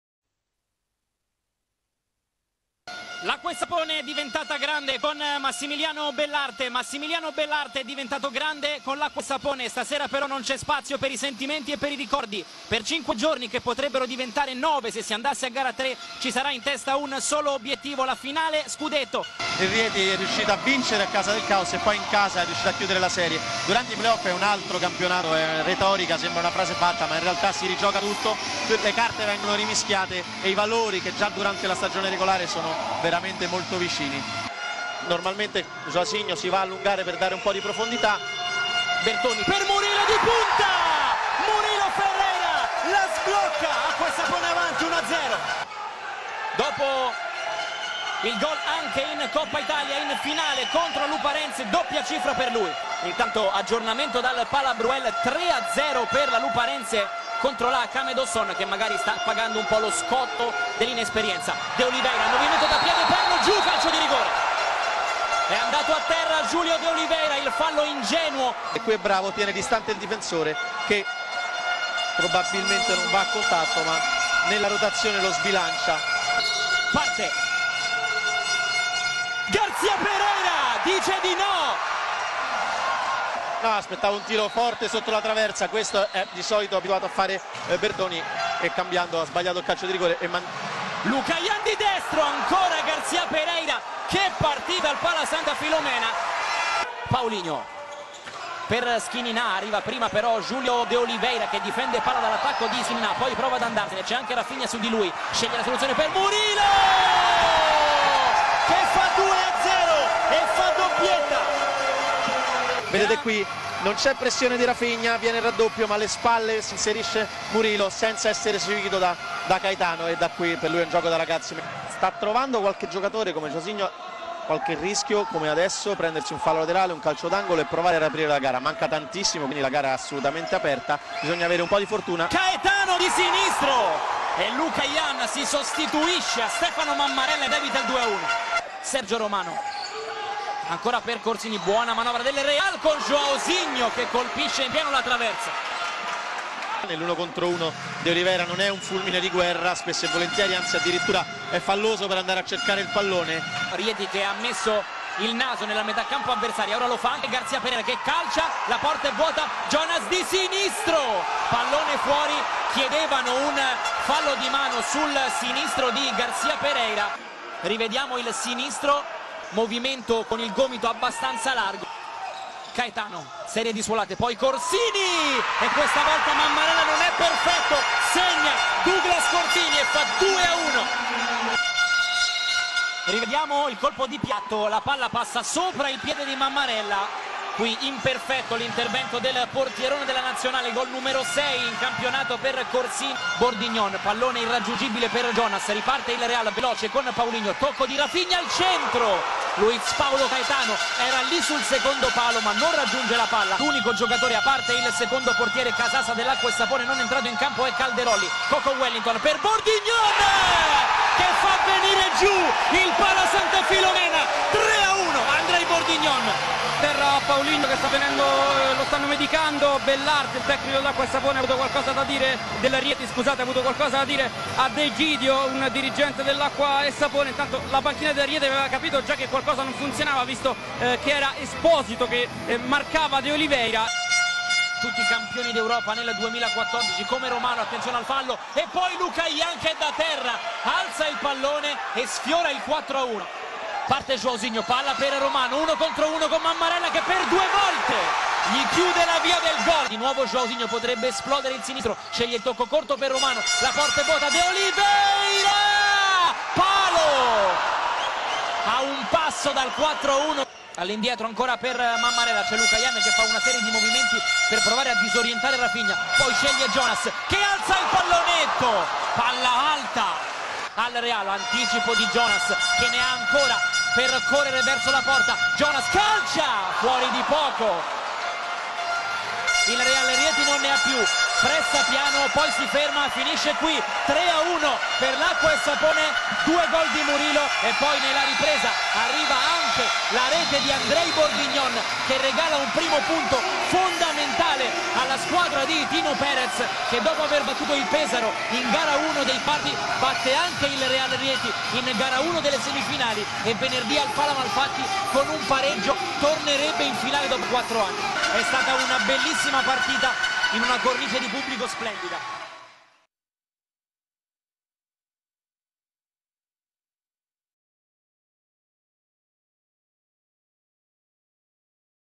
Italy League - Semi Finals / 1st Leg Match - Acqua&Sapone Unigross 3x1 Real Rieti
في أولى مباريات نصف النهائي للدوري الإيطالي للفوتسال, أكوا سابوني يهزم ريال ريتي و بنتيجة 3-1 في مباراة قوية نقدم لكم ملخصها...